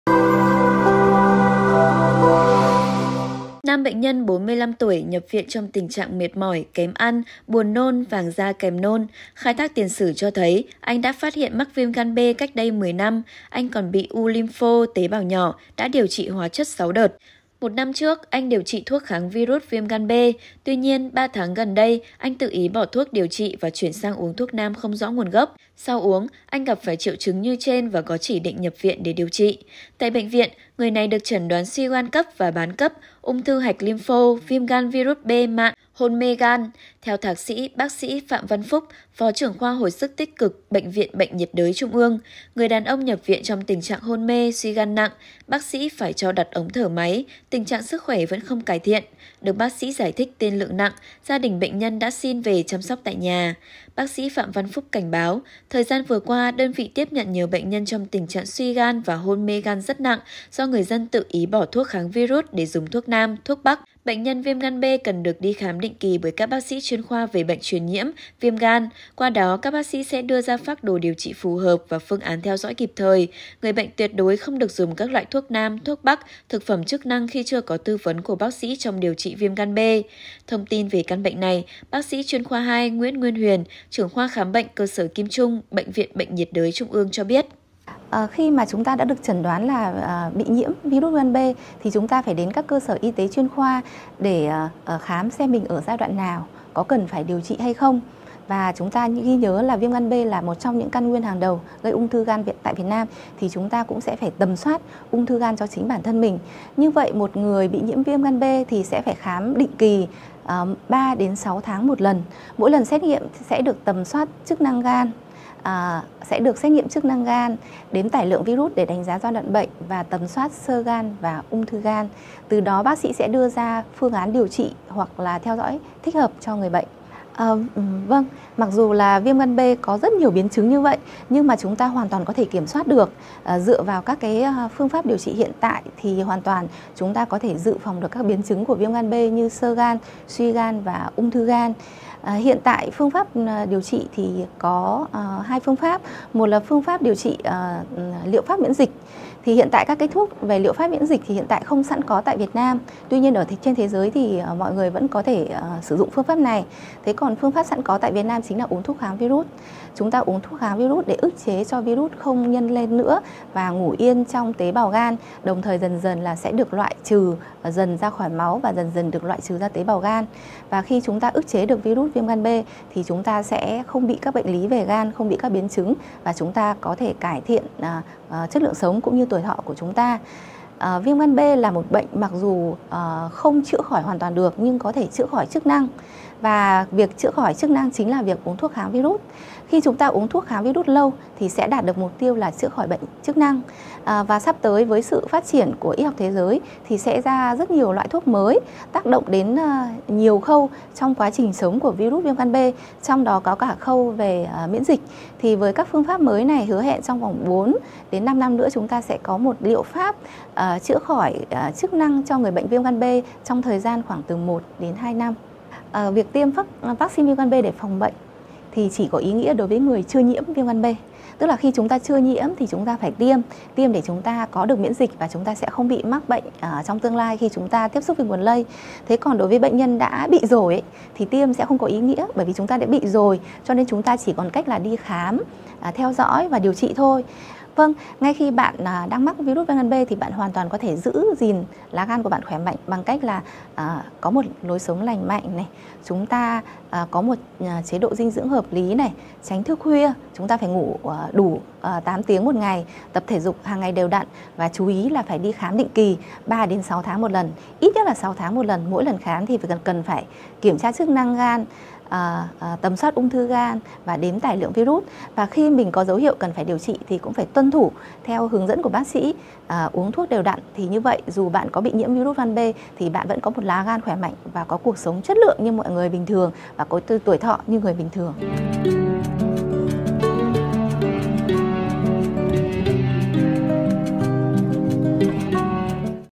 Tự Uống Thuốc Nam Chữa Viêm Gan B, Người Đàn Ông Cấp Cứu Vì Suy Gan Nặng, Tiên Lượng Xấu I SKĐS
SKĐS | TNam bệnh nhân 45 tuổi, nhâp viện trong tình trạng mệt mỏi, kém ăn, buồn nôn, vàng da kèm theo nôn. Khai thác tiền sử cho thấy, anh phát hiện mắc viêm gan B cách đây 10 năm. Anh còn bị u lympho tế bào nhỏ đã điều trị hóa chất 6 đợt.

Mời quý vị và các bạn xem thêm: Nghe Tới Án Tử Hình, Bà Trương Mỹ Lan Bất Ngờ Ngất Xỉu Tại Tòa